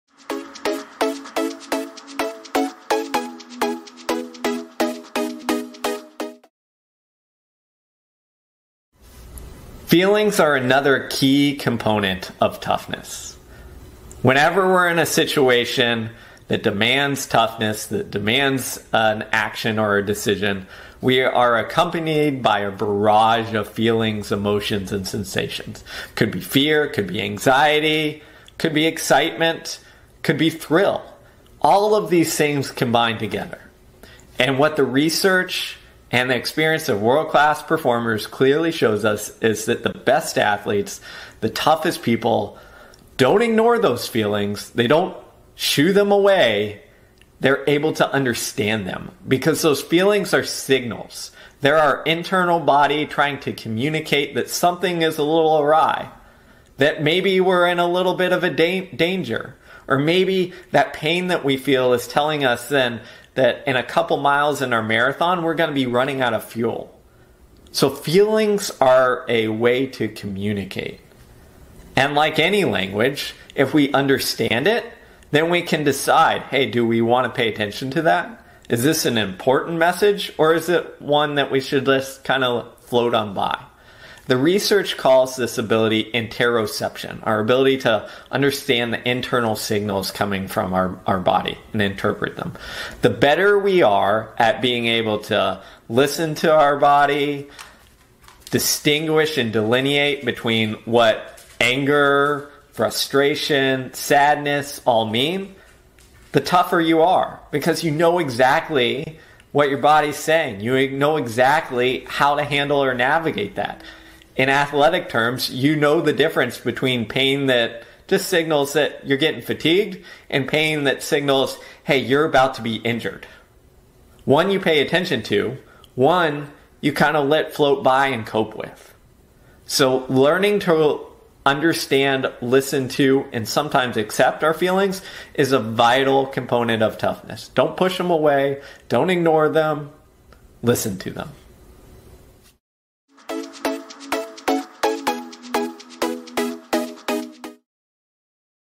Our Feelings and Emotions Are Information. Learn to Listen to them.
We're often told to ignore our feelings and emotions. The latest science tells us the opposite. Our feelings are information. If we learn to listen to them, our performance will improve.